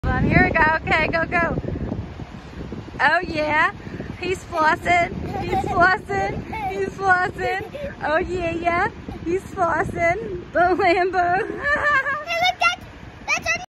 “Flossing” in front of the lambo. Huge gear head here this kids gets so excited for cool cars!!
Flossing and dancing silliness outside Wilde Lamborghini